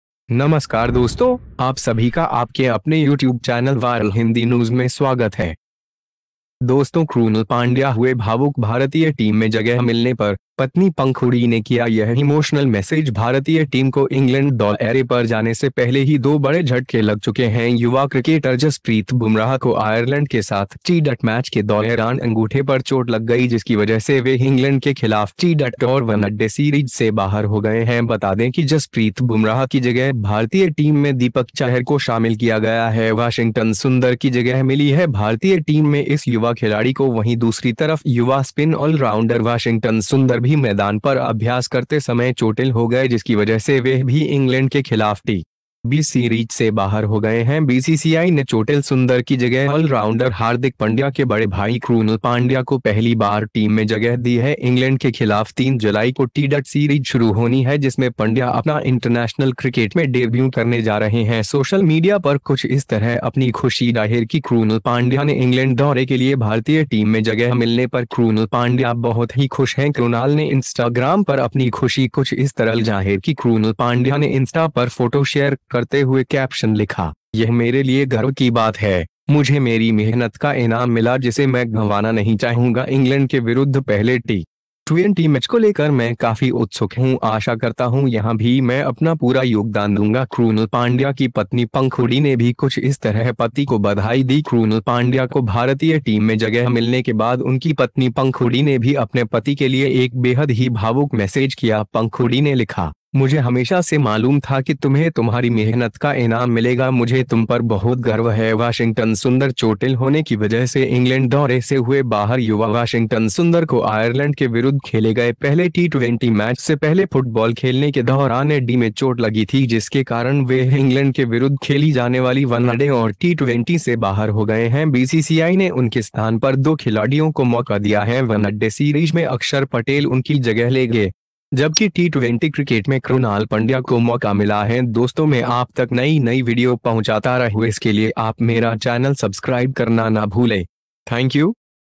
Krunal Pandya हुए भावुक भारतीय टीम में जगह मिलने पर, Wife Pankhudi ने किया यह emotional message
on receiving the place in the emotional indian team krunal pandya Wife Pankhudi did this emotional message
भारतीय टीम को इंग्लैंड दौैरे पर जाने से पहले ही दो बड़े झटके लग चुके हैं। युवा क्रिकेटर जसप्रीत बुमराह को आयरलैंड के साथ टी-20 मैच के दौैरान अंगूठे पर चोट लग गई जिसकी वजह से वह इंग्लैंड के खिलाफ टी-20 और वनडे सीरीज से बाहर हो गए हैं।